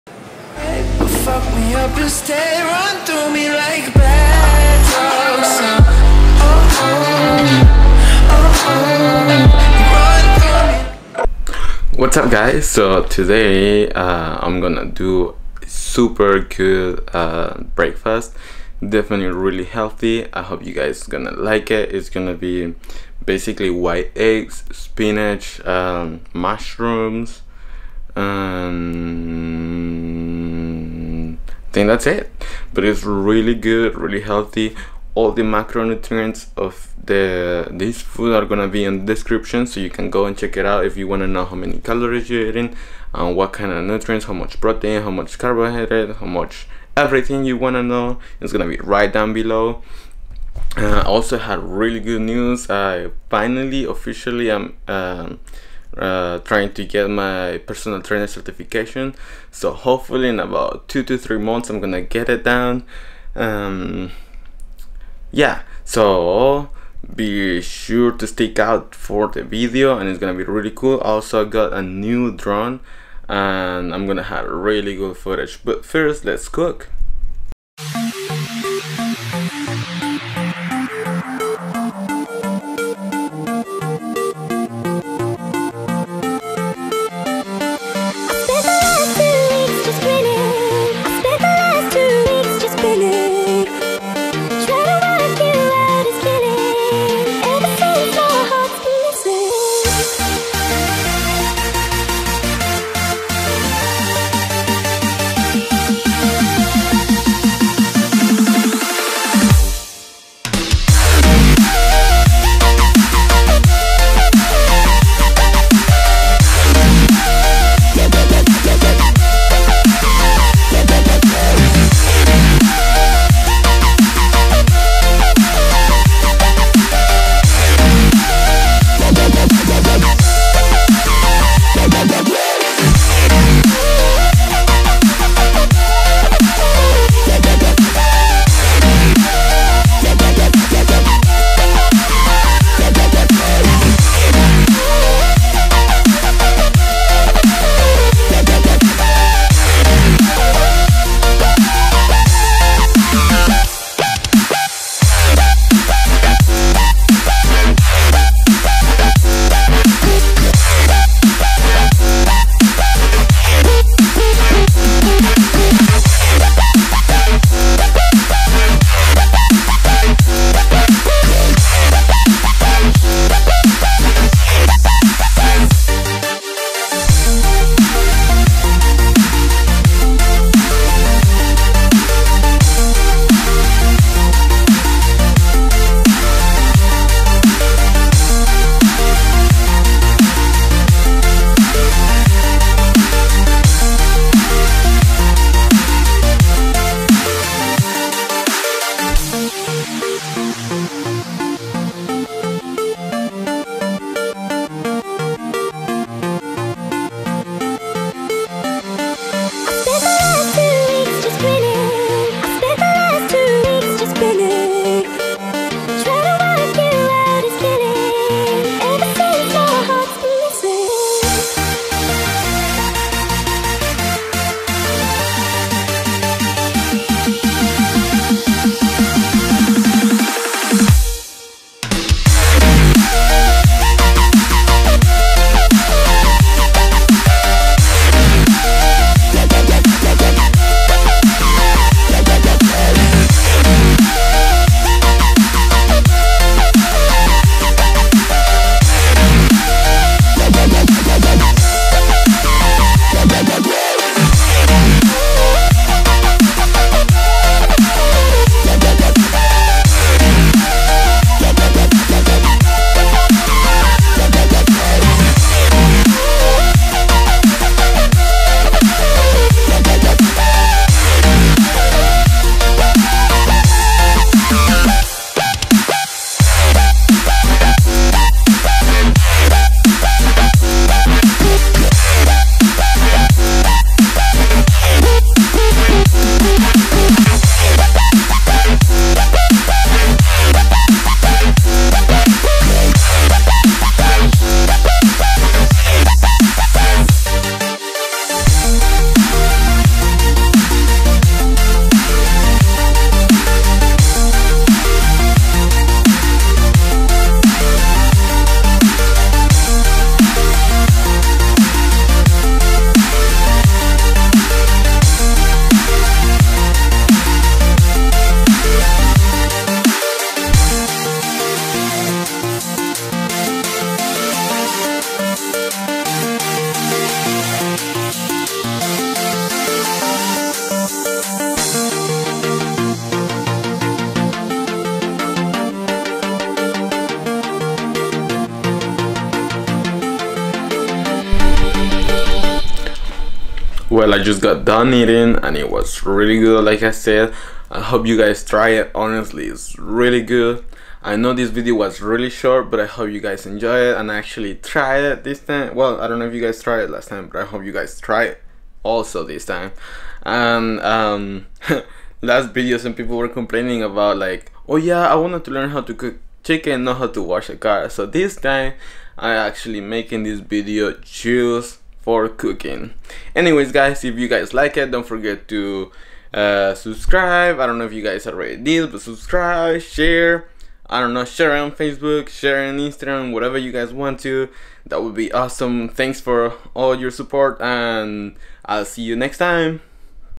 THE BEST BREAKFAST WHEN YOU ARE DIETING!! (KETO FRIENDLY)
White eggs: 25 Cal. Serving size 3 Tbs.
Avocado oil: 130 Cal. Serving size 1Tbs.
Mozzarella Cheese: 20 Cal. Serving size 1/4 Cup.
Spinach: 80 Cal.  Serving Size 1 Cup.